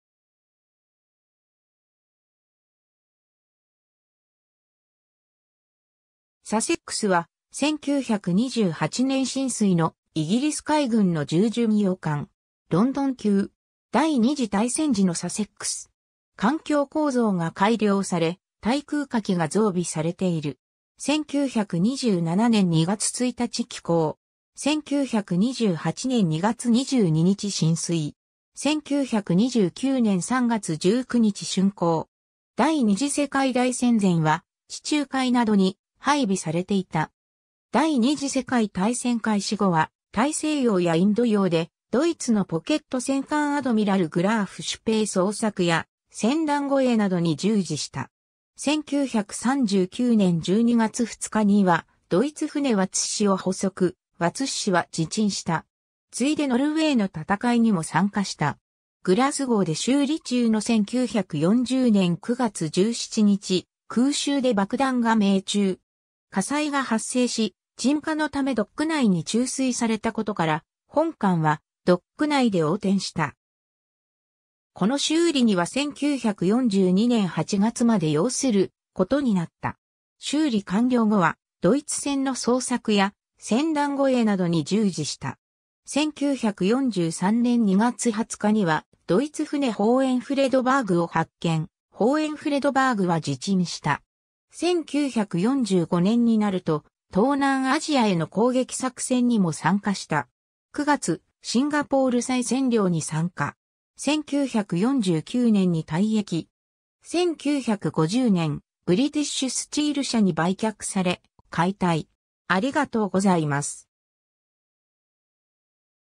サセックス (重巡洋艦)
サセックス (HMS Sussex, 96) は、1928年進水のイギリス海軍の重巡洋艦。
ロンドン級。
 第二次大戦時のサセックス。
艦橋構造が改良され、対空火器が増備されている。
 1927年2月1日起工。
1928年2月22日進水。
1929年3月19日竣工。
 第二次世界大戦前は地中海などに配備されていた。
 第二次世界大戦開始後は、大西洋やインド洋で、ドイツのポケット戦艦アドミラル・グラーフ・シュペー捜索や、船団護衛などに従事した。
1939年12月2日にはドイツ船Watussiを捕捉、Watussiは自沈した。
 次いでノルウェーの戦いにも参加した。
 グラスゴーで修理中の1940年9月17日、空襲で爆弾が命中。
火災が発生し、鎮火のためドック内に注水されたことから本艦はドック内で横転した。
この修理には1942年8月まで要することになった。
 修理完了後は、ドイツ船の捜索や船団護衛などに従事した。
1943年2月20日にはドイツ船Hohenfriedbergを発見、Hohenfriedbergは自沈した。
 1945年になると、東南アジアへの攻撃作戦にも参加した。
9月、シンガポール再占領に参加。
 1949年に退役。
1950年、ブリティッシュ・スチール社に売却され、解体。